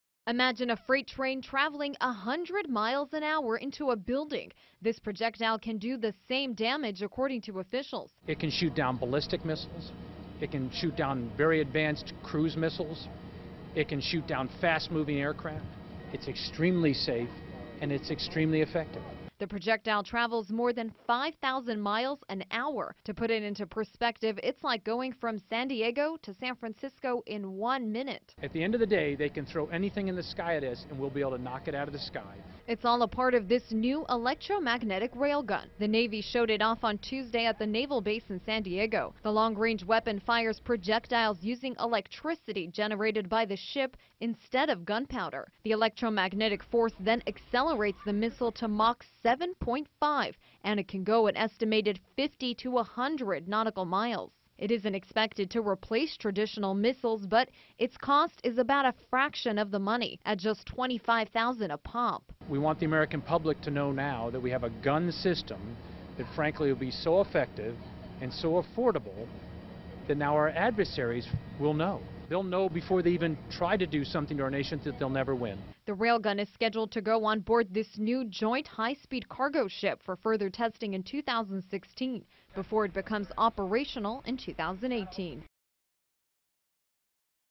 Electromagnetic Mach 7.5 rail gun
Electromagnetic rail gun built by European bae for the US Navy.